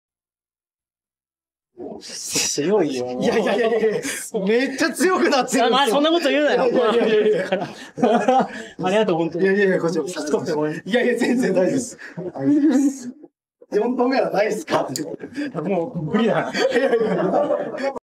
堀口恭司に3連敗してリベンジ戦を諦める総合格闘家【RIZIN40/扇久保博正】
堀口恭司に4度目の対戦を問われ即答で辞退しようとする扇久保博正。